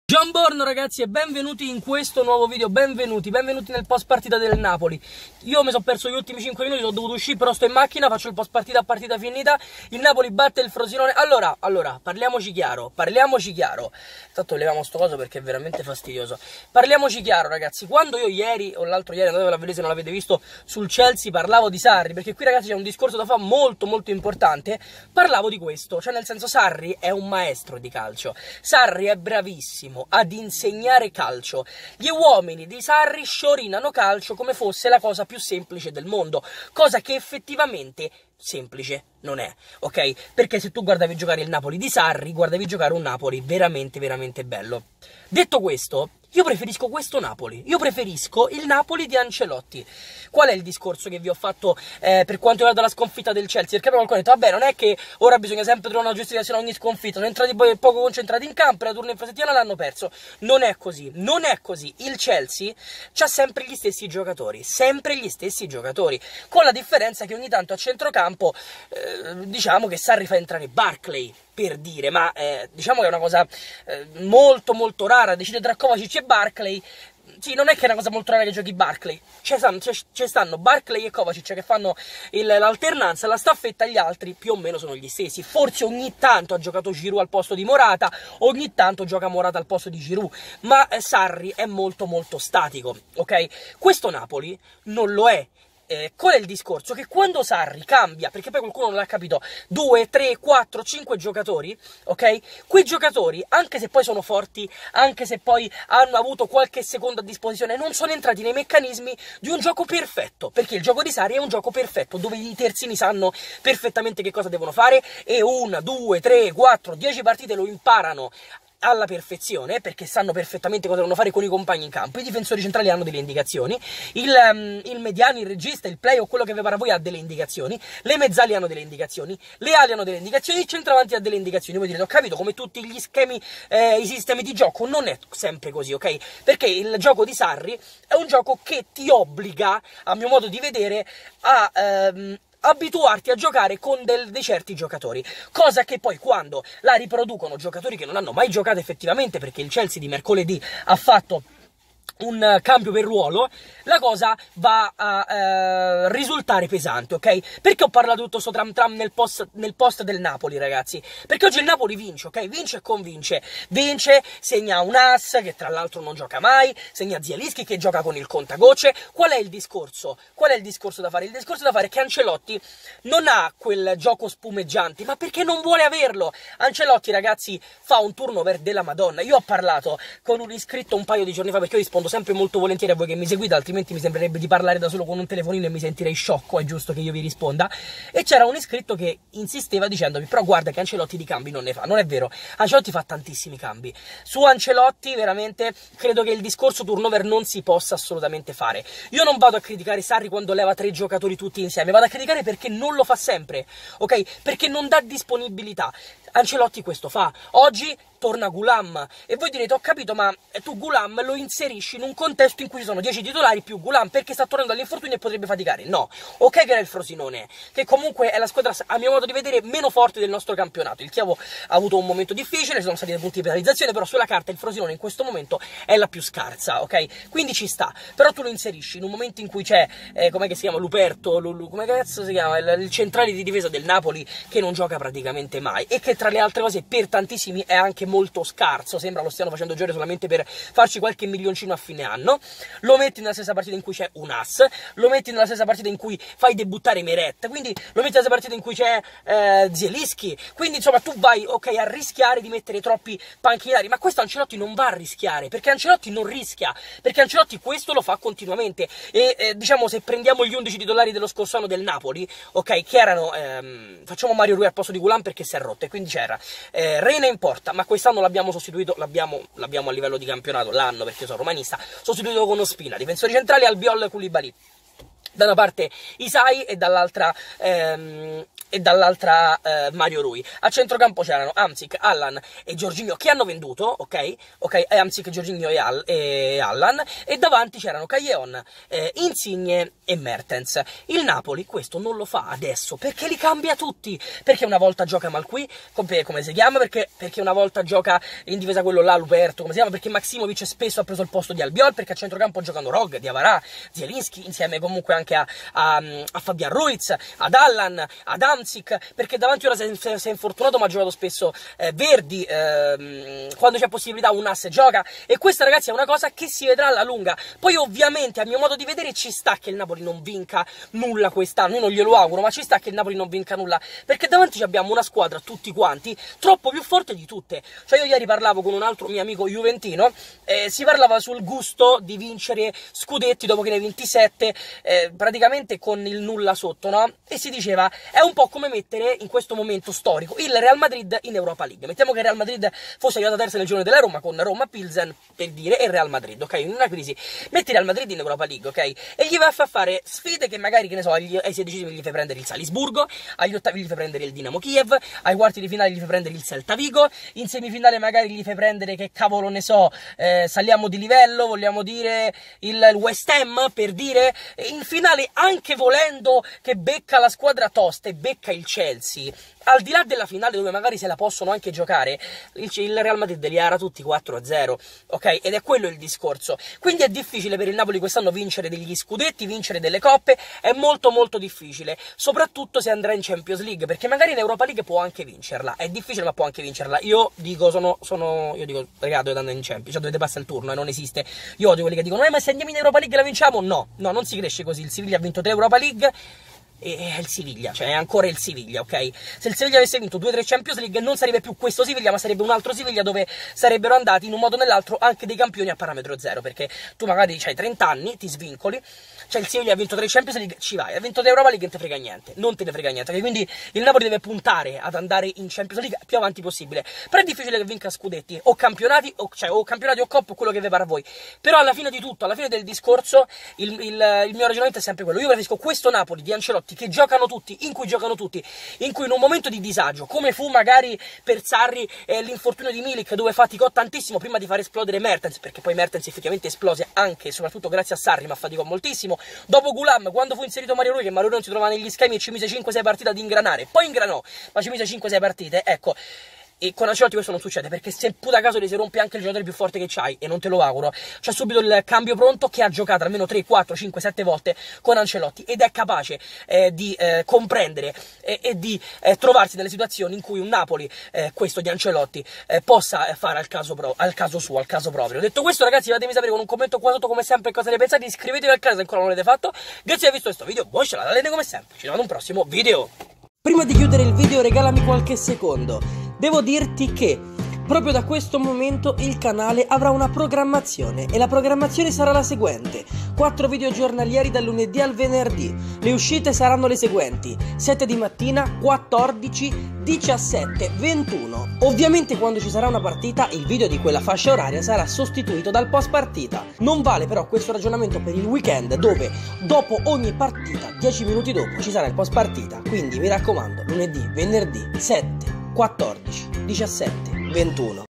QUESTO NAPOLI DI ANCELOTTI È DIECI VOLTE MEGLIO DI QUELLO DI SARRI - Napoli-Frosinone 4-0
Parliamo di questo Napoli nella vittoria di oggi contro il Frosinone.  A me piace un sacco questo, con tanti calciatori a girare ma tutti indispensabili.